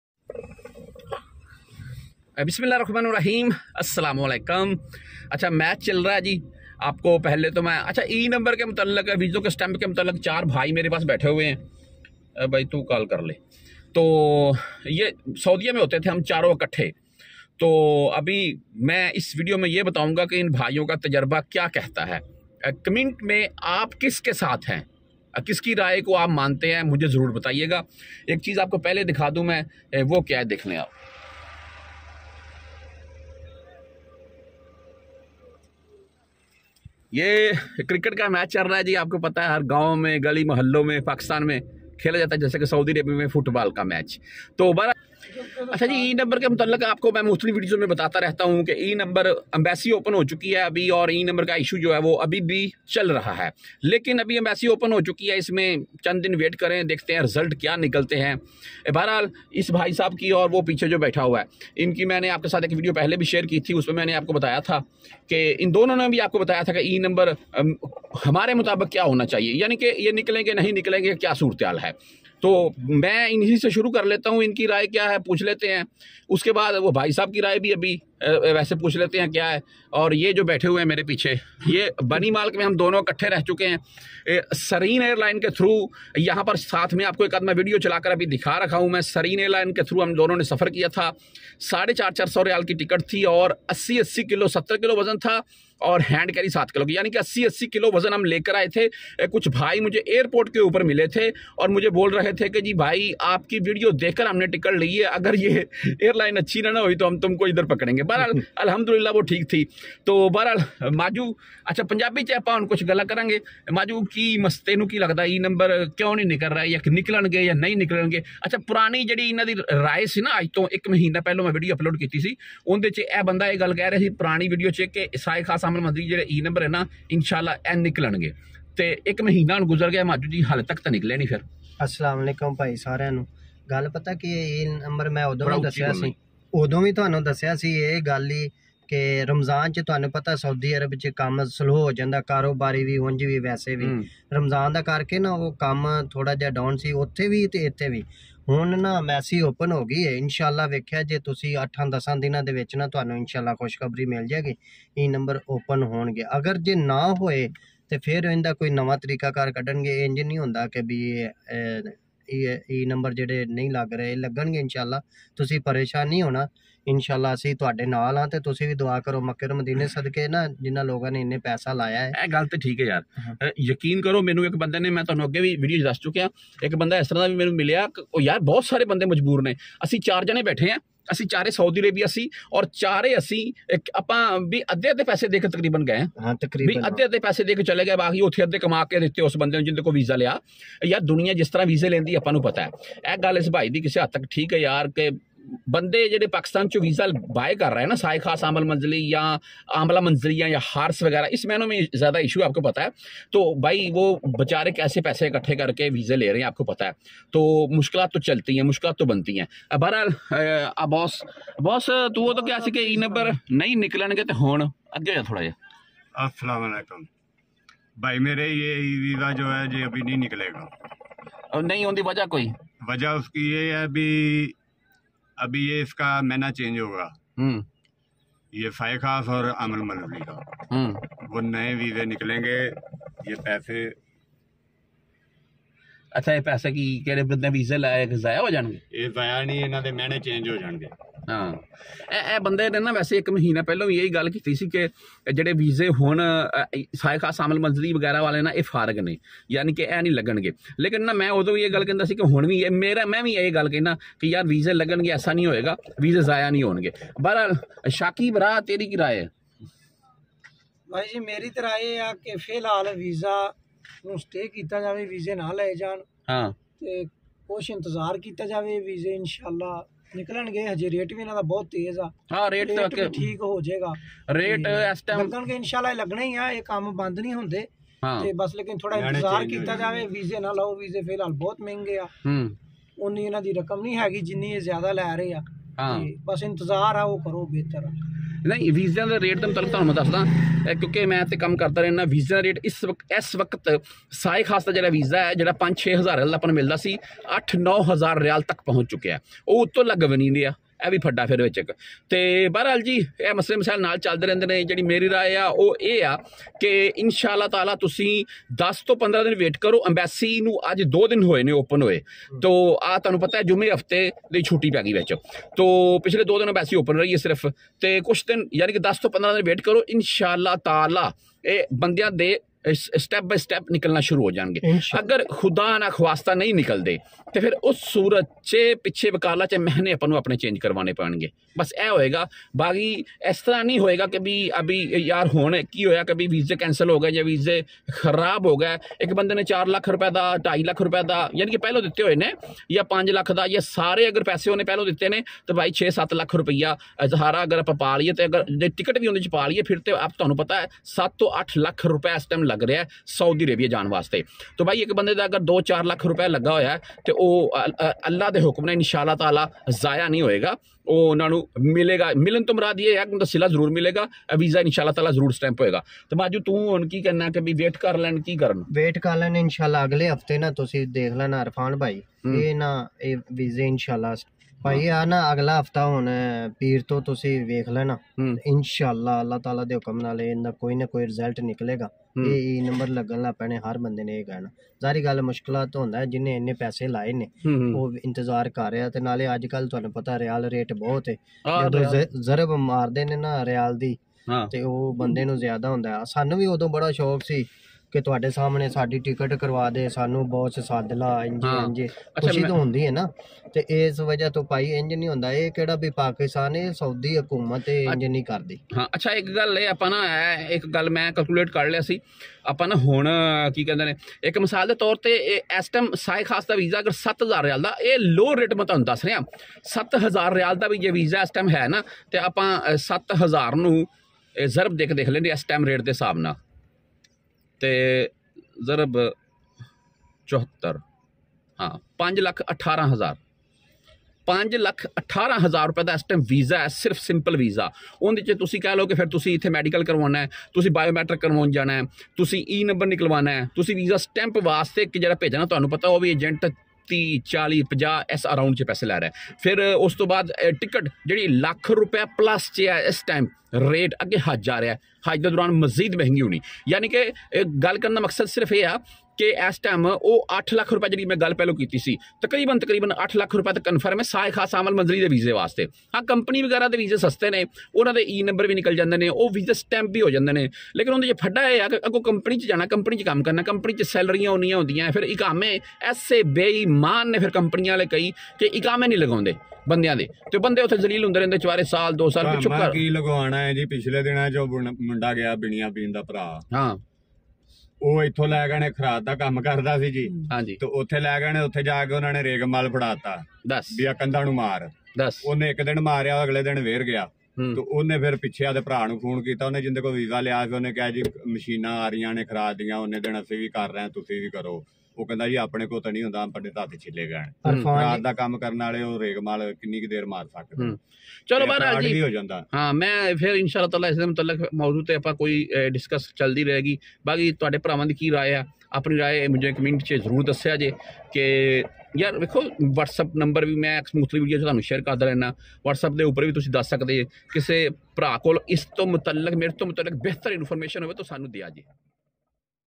Discussing Saudi Visas with Friends from Saudi Arabia | Visa E Number Latest News,
Saudi Arabia Latest Update News today in Urdu and Hindi, Saudi News in Urdu, Today News in Saudi Arabia, Discussing Saudi Visas with Friends from Saudi Arabia | Visa E Number Latest News,